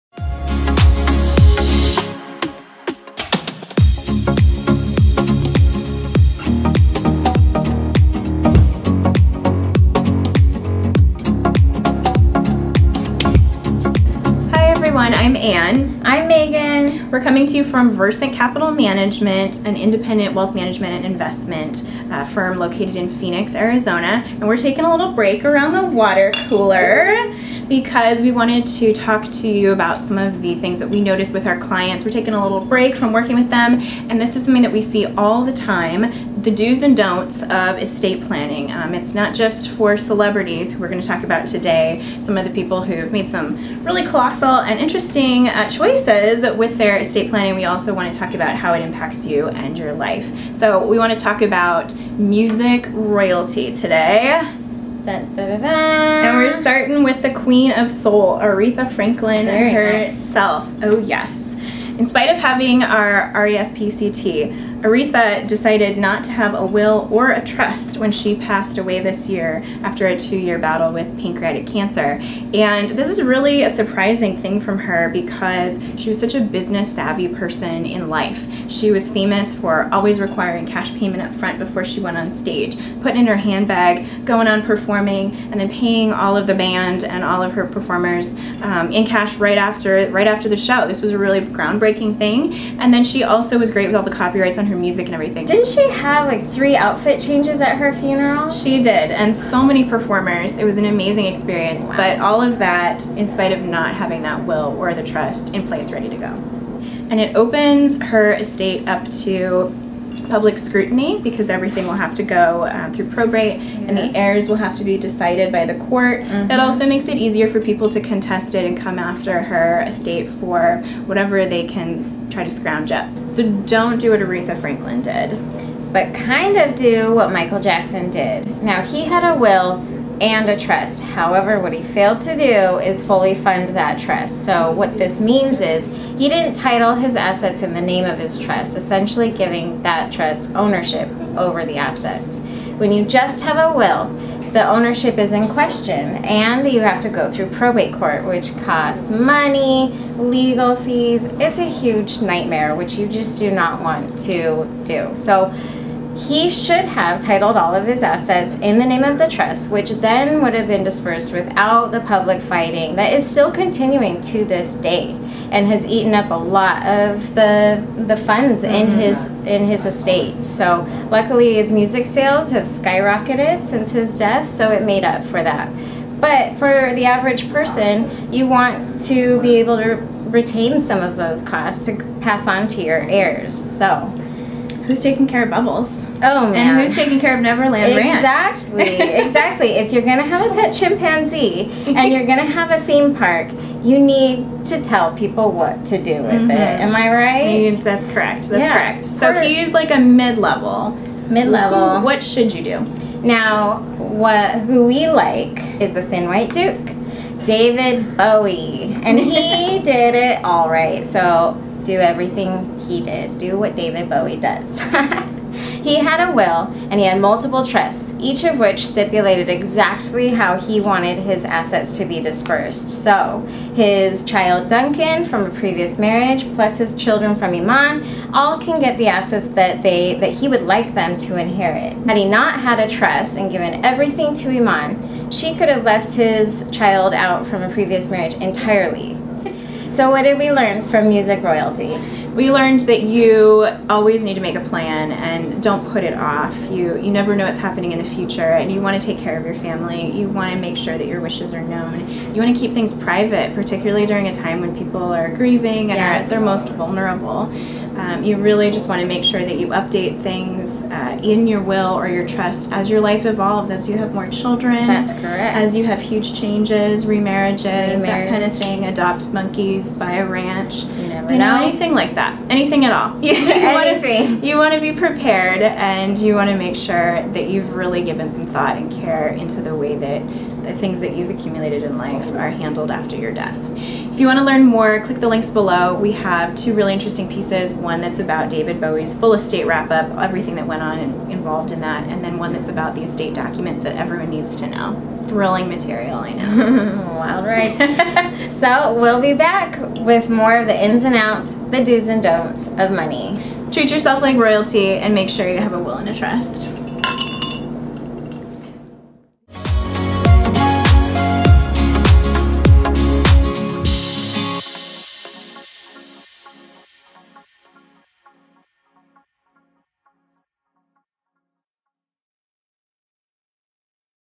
Water Cooler Talk: Episode 1
The dos and don'ts, and ins and outs of finance.  Episode one discusses estate planning mistakes that celebrities have made, and how to avoid them.

To read more about David Bowie's estate plans
To read about the estate documents that everyone should have